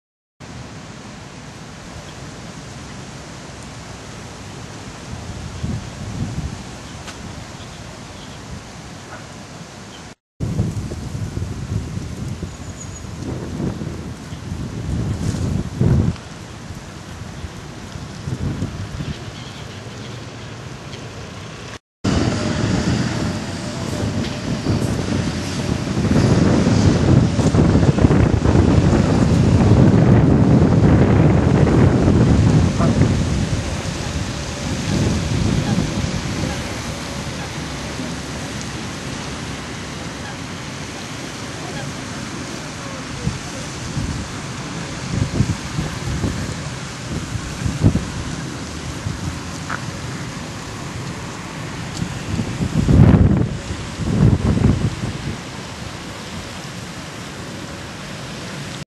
Victoria Park West Lake
This traditional Victorian park is just over two miles east of the Bank of England in the City of London.